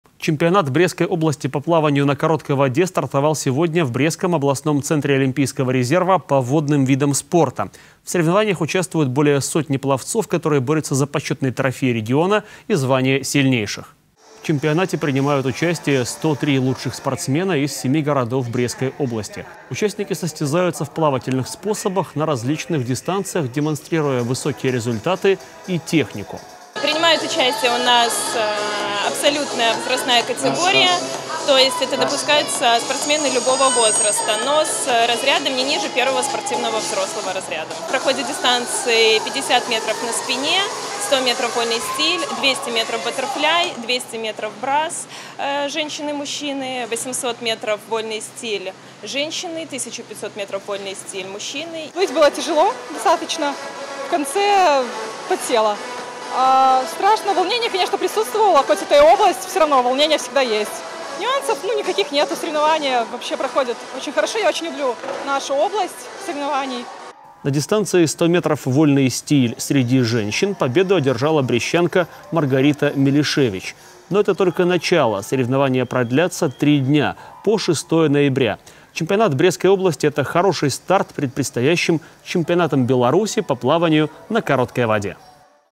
ЧЕМПИОНАТ БРЕСТСКОЙ ОБЛАСТИ ПО ПЛАВАНИЮ
Чемпионат Брестской области по плаванию на короткой воде стартовал сегодня в Брестском областном центре олимпийского резерва по водным видам спорта. В соревнованиях участвуют более сотни пловцов, которые борются за почётный трофей региона и звание чемпиона.